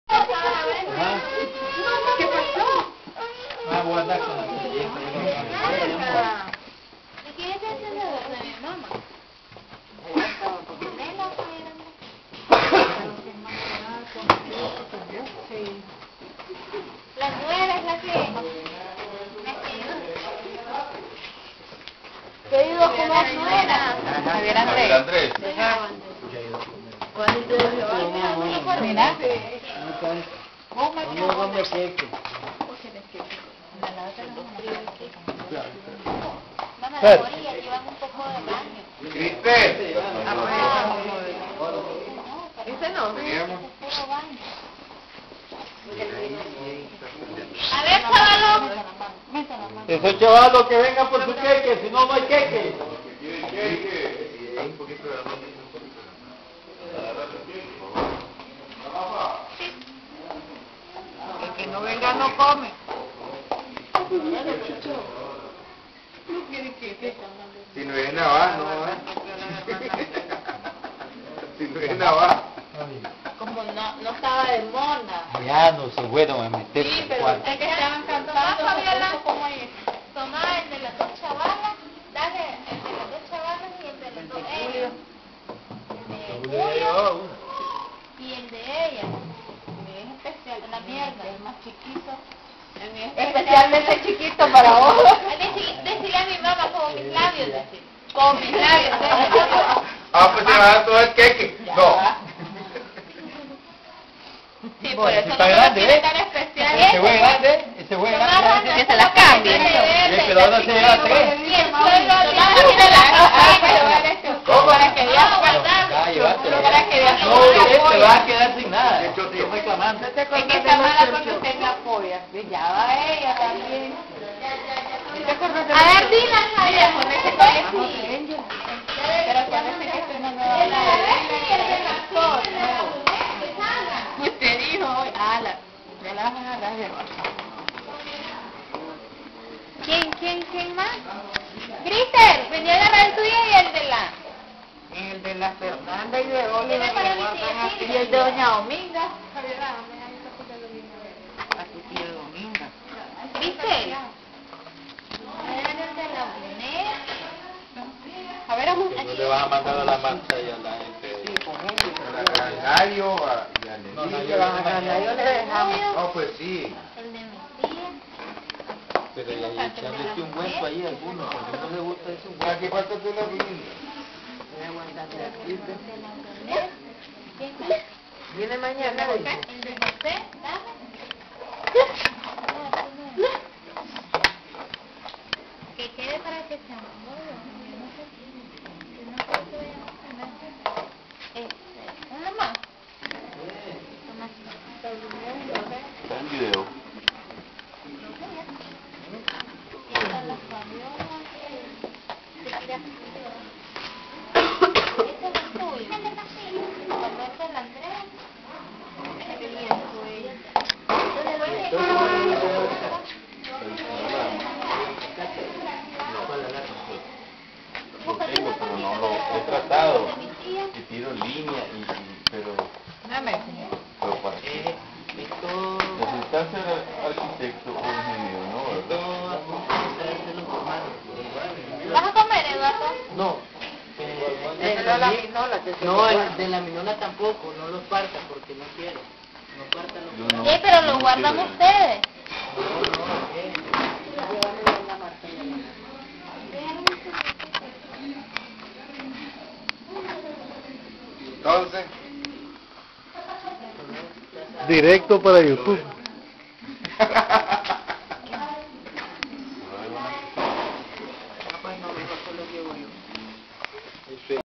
Cumpleaños Jeanine Vallecillo
Celebración familiar del cumpleaños número 50 de Jeanine Vallecillo.